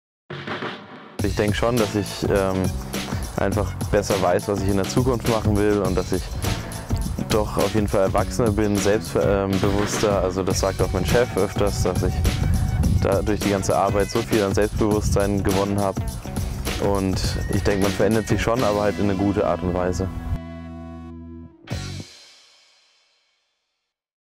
Miteinander Freude Leben / Clip 8
Miteinander Freude Leben - die Freiwilligendienste der Diakonie Bayern. Warum man das machen sollte, erfährst Du hier.
Und mehr über die Dienste der Diakonie Bayern gibt es